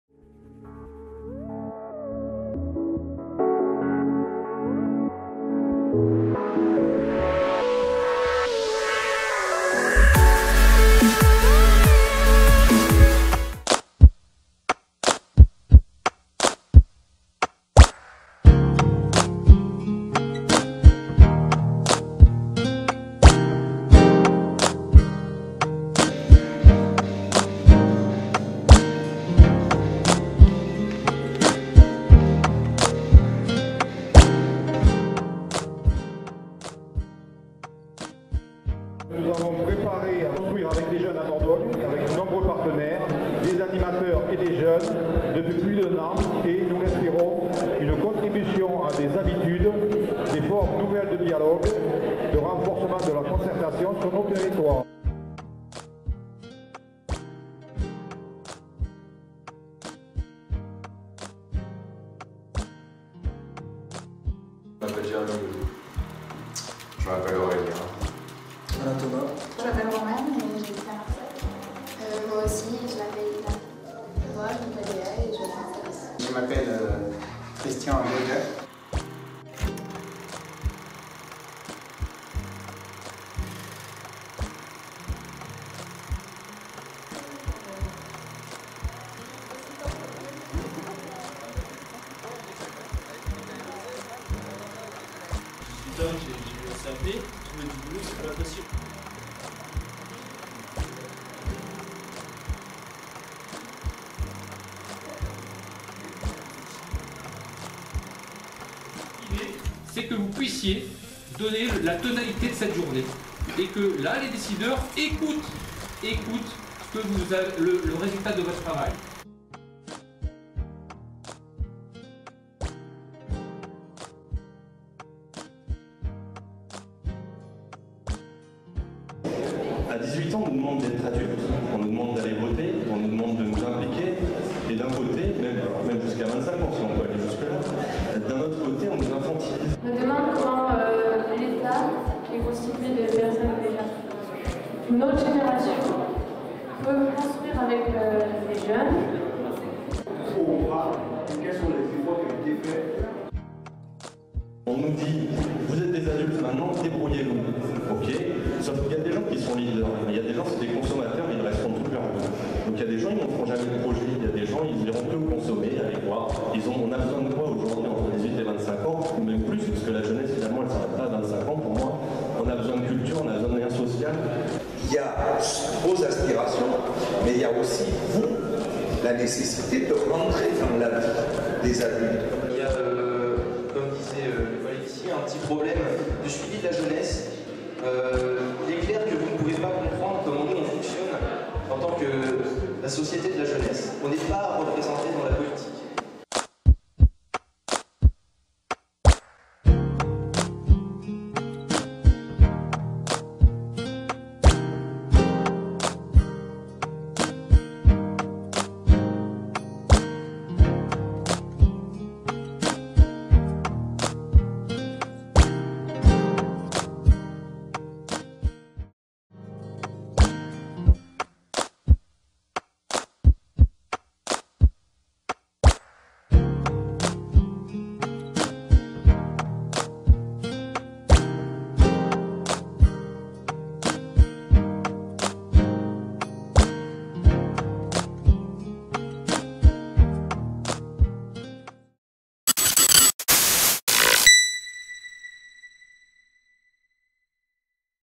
Construire avec les jeunes en Dordogne -Samedi 15 Novembre 2014-
Voici le compte rendu de la journée du 15 Novembre 2015 au centre de la communication de Périgueux, lors de la rencontre Construire avec les jeunes en Dordogne